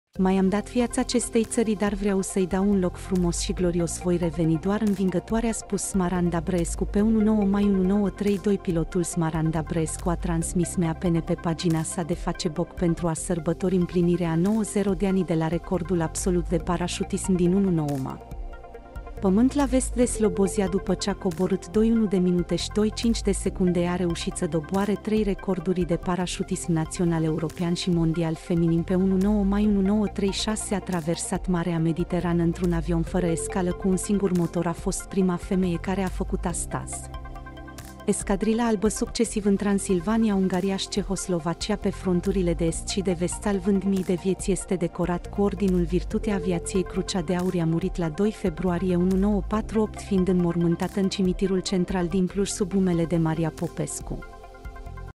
Smaranda Brăescu, prima femeie pilot și parașutist brevetat din România
Ministerul Apărării aduce un omagiu unei figuri feminine importante din România.
"Mi-am dat viața acestei țări, dar vreau să-i dau un loc frumos și glorios. Voi reveni doar învingătoare", a spus Smaranda Brăescu.

Pe 19 mai 1932, pilotul Smaranda Brăescu a transmis MApN pe pagina sa de Facebook pentru a sărbători împlinirea a 90 de ani de la recordul absolut de parașutism din 19 mai 1932, iar 2022 este dedicat Smarandei Brăescu.

Smaranda Brăescu s-a născut la 21 mai 1897 în satul Hănţeşti, iar activităţile sale sunt de neuitat.

Pe 5 iulie 1928 a parașut pentru prima dată de la 600 de metri.

În dimineața zilei de 2 octombrie 1931, Smaranda Brăescu a sărit de la altitudinea de 6.000 de metri și a ajuns la pământ la vest de Slobozia după ce a coborât 21 de minute și 25 de secunde. Ea a reușit să doboare trei recorduri de parașutism: național, european și mondial feminin.

Pe 19 mai 1936, a traversat Marea Mediterană într-un avion fără escală cu un singur motor. A fost prima femeie care a făcut asta. Zborul operează pe ruta Roma - Tripoli și durează aproximativ 6 ore și 45 de minute.

Ea a servit ca instructor la Batalionul 1 Parașutiști din Băneasa și a fost unul dintre puținii instructori de parașutiști militari la acea vreme.

În timpul celui de-al Doilea Război Mondial, a slujit ca pilot în celebra aeronave medicale „Escadrila Albă”, succesiv în Transilvania, Ungaria și Cehoslovacia pe fronturile de Est și de Vest, salvând mii de vieți. Este decorat cu Ordinul Virtutea Aviației - Crucea de Aur.

Ea a murit la 2 februarie 1948, fiind înmormântată în cimitirul central din Cluj sub numele de Maria Popescu.